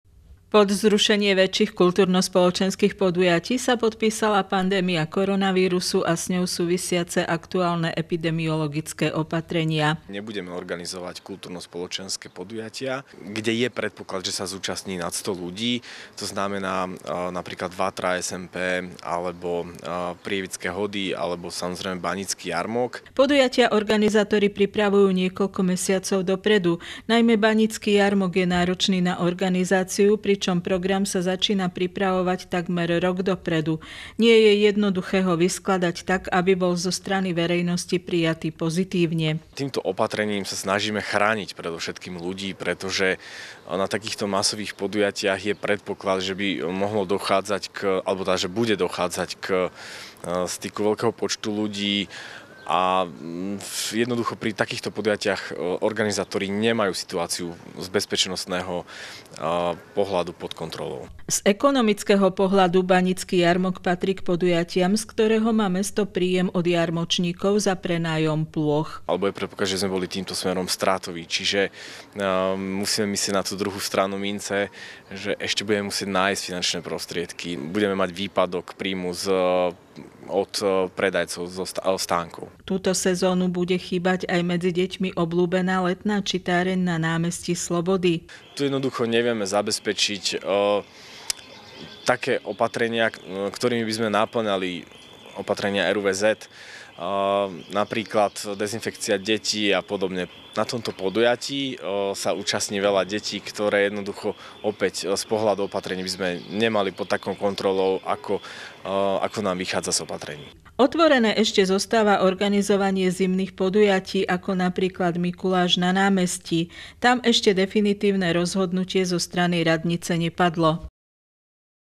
Banícky jarmok nebude
Letné a jesenné kultúrno-spoločenské podujatia organizované mestom Prievidza v tomto roku nebudú. Medzi nimi je i tradičný  Banícky jarmok, ktorý do okresného mesta každoročne priláka tisícky Hornonitranov.